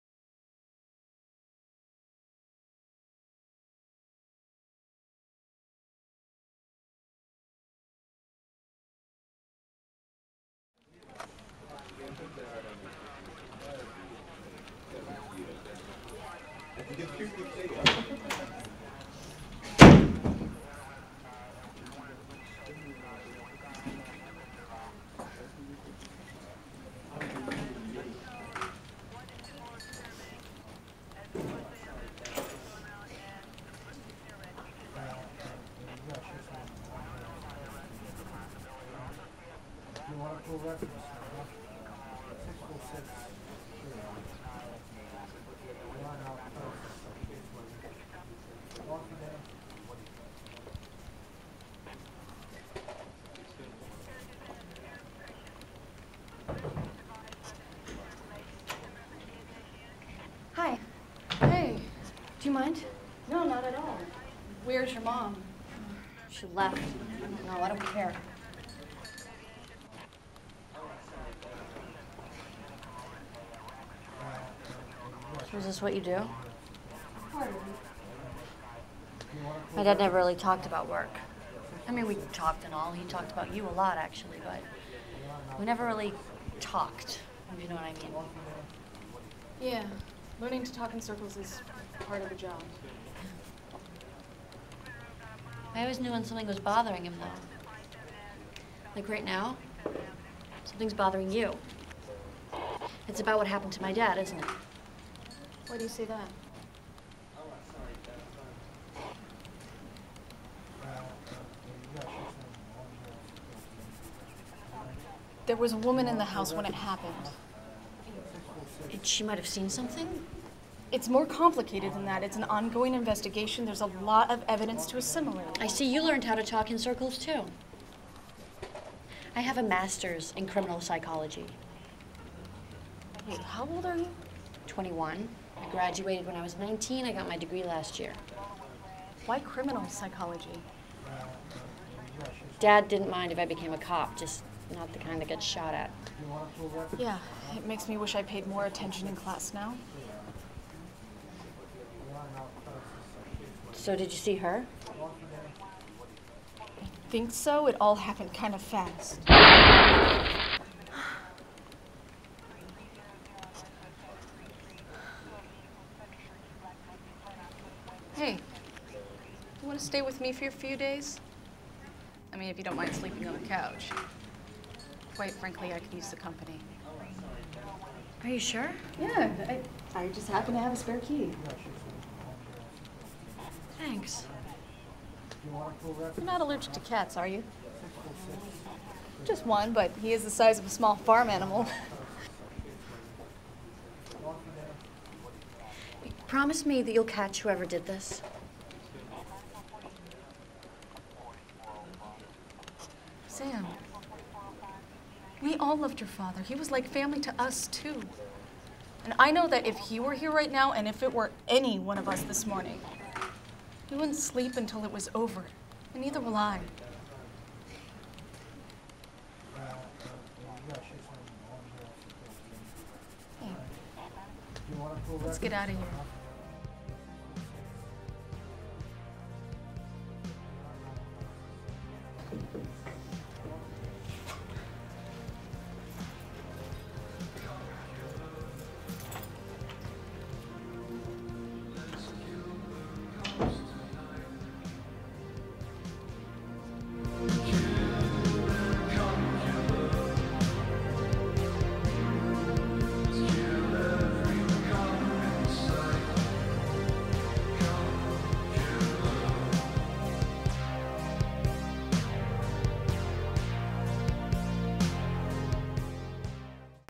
Cop Killer - a scene
Cop Killer is a scene that is a direct outgrowth of the Sport of Acting class.